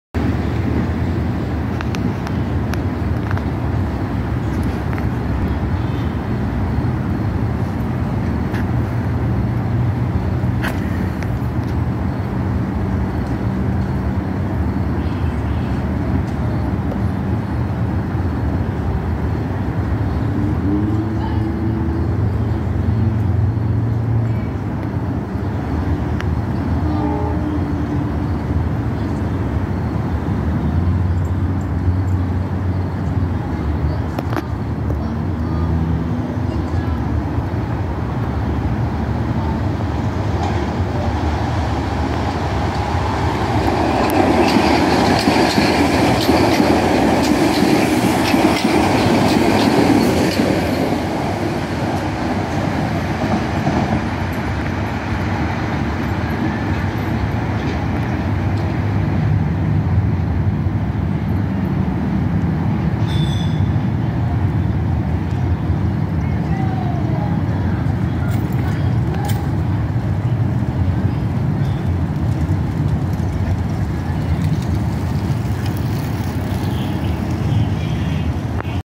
Metro north: Southeast M3 train #9647 above 204th street bridge!
shown here in this video is metro north train 9647 the 5:09pm Harlem line express to Southeast using a 6 car set of M3As while M3As on metro north often run on weekends mostly 8 car ones do 6 car M3As don't often run on weekends so this was interesting to see this video was taken Sunday September 11th 2022 at 5:29pm